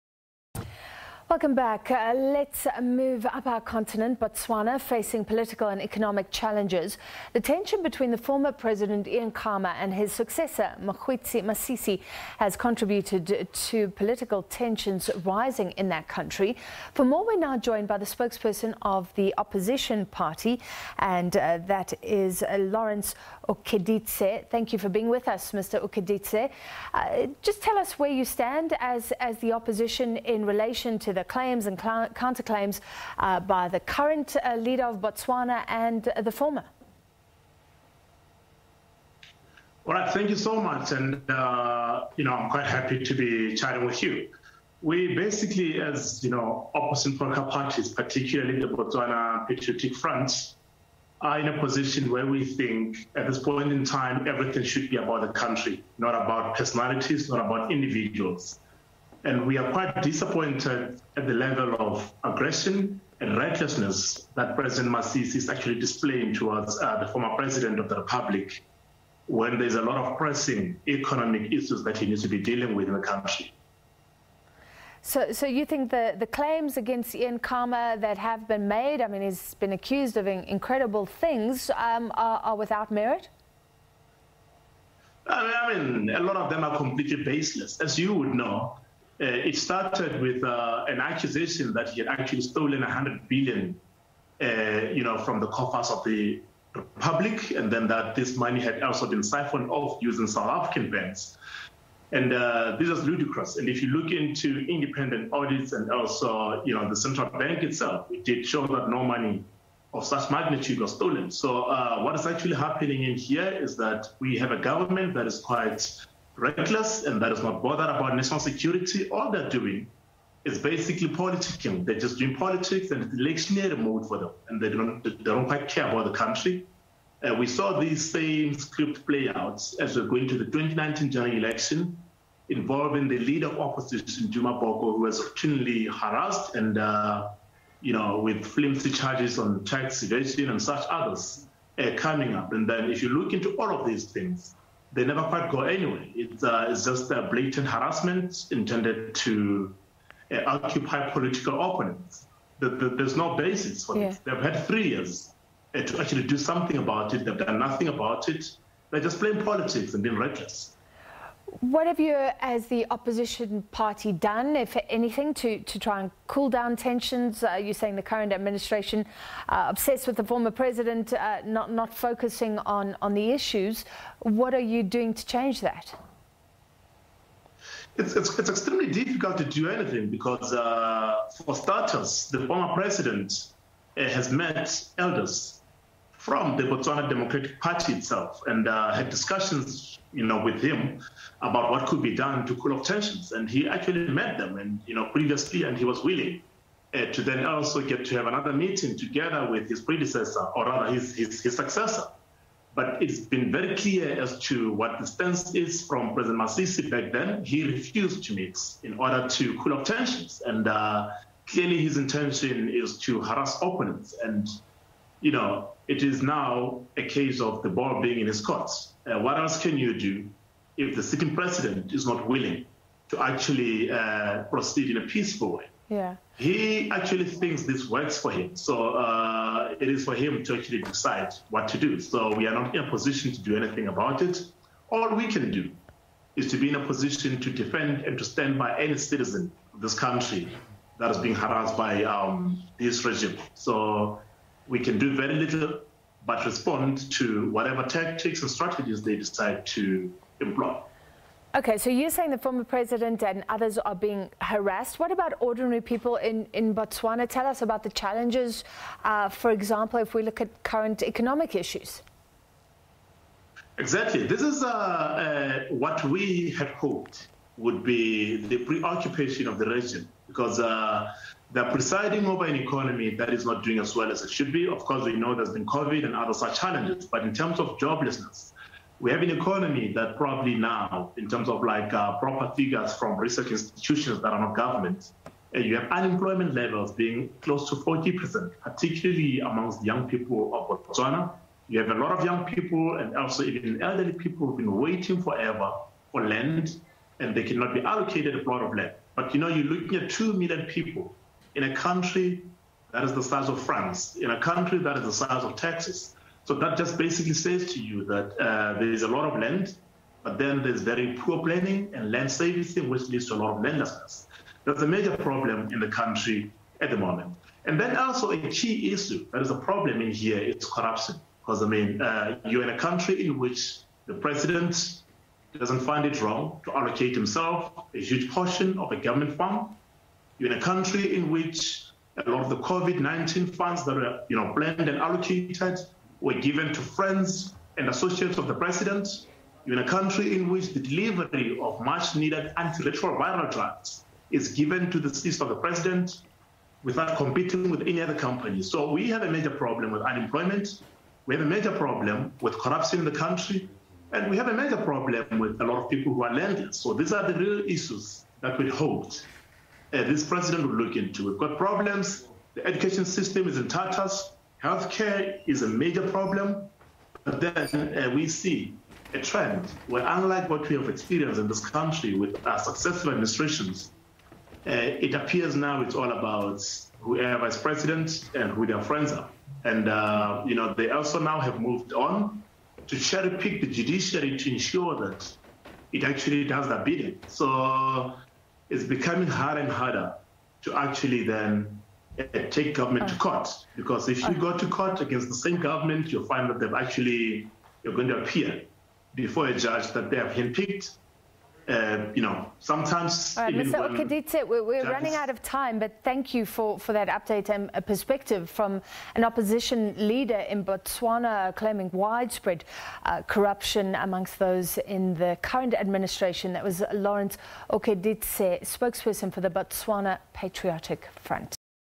Update on political tensions between Botswana's Khana and Masisi: Lawrence Ookeditse
Botswana is facing political and economic challenges. The tension between the Former President Ian Khama and his successor Mokgwetsi Masisi has contributed to the the political tension in that country. For more we are joined but the spokesperson of the opposition party.